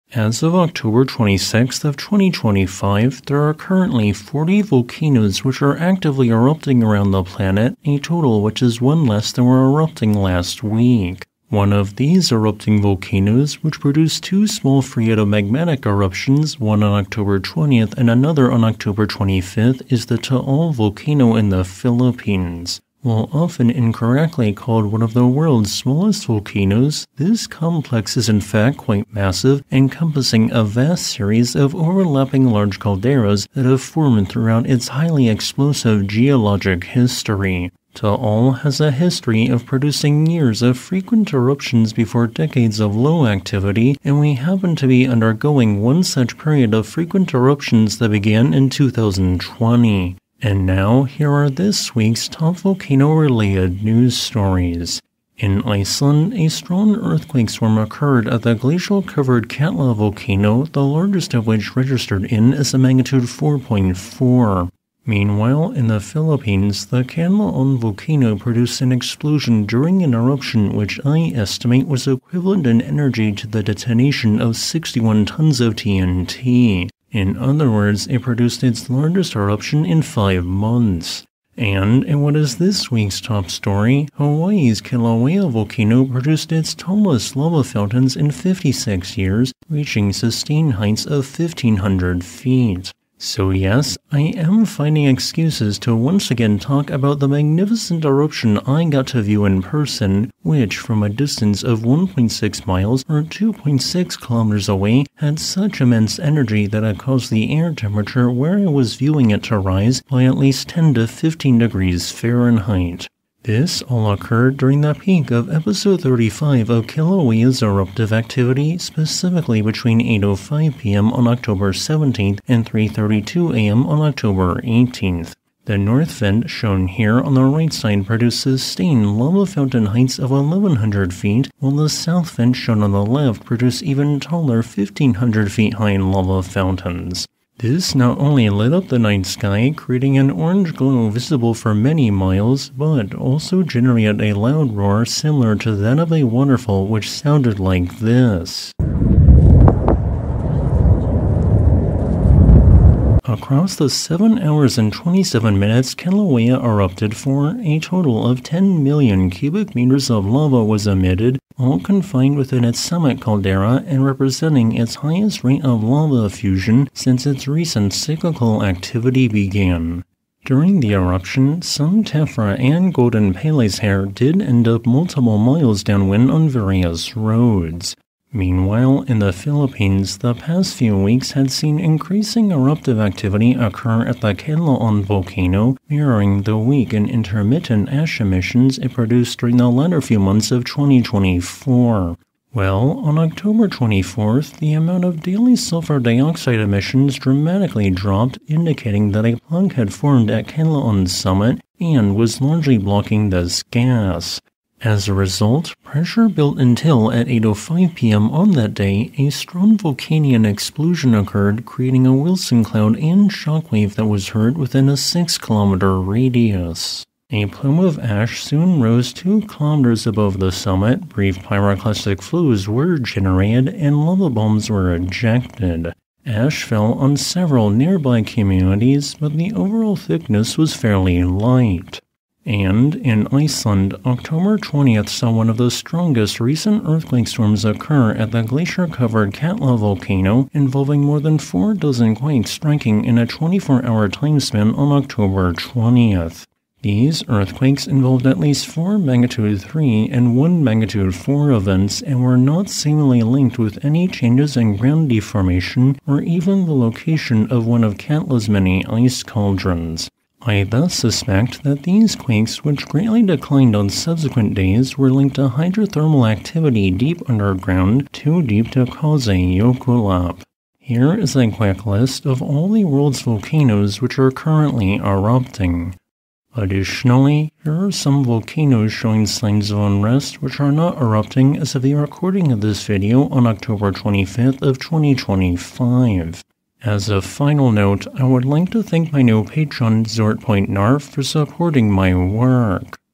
This Week in Volcano News; Kanlaon Erupts, Kilauea's 1,500 Ft Lava Fountains
The Kanlaon volcano explosively erupted, creating a visible pressure wave and generating pyroclastic flows. Meanwhile, in Hawaii, a series of once in a generation 1,500 foot high sustained lava fountains erupted from Kilauea. And, in Iceland, an unusual earthquake swarm occurred at the glacier covered Katla volcano. Today's video will discuss these stories through the opinion and analysis of a geologist and list the 40 world volcanoes that are actively erupting.

This video's thumbnail image shows an October 17th 2025 image of Kilauea erupting.
Note: Do not go within the 4 kilometer exclusion zone surrounding all directions of Kanlaon's summit.

Sources/Citations:
[1] Phivolcs
[4] CIVISA
[5] IVAR (Instituto de Investigação em Vulcanologia e Avaliação de Riscos)
[6] Sernageomin
[7] Vanuatu Meteorology & Geo-Hazards Department
[8] Servicio Geológico Colombiano
[9] Instituto Geofísico Escuela Politécnica Nacional
[20] Observatorio Vulcanológico y Sismológico de Costa Rica (OVSICORI)

0:00 Taal Erupts
0:50 This Week's Top Stories
1:22 Kilauea Eruption
2:58 Kanlaon Eruption
3:52 Katla Earthquake Swarm
4:30 List of Erupting Volcanoes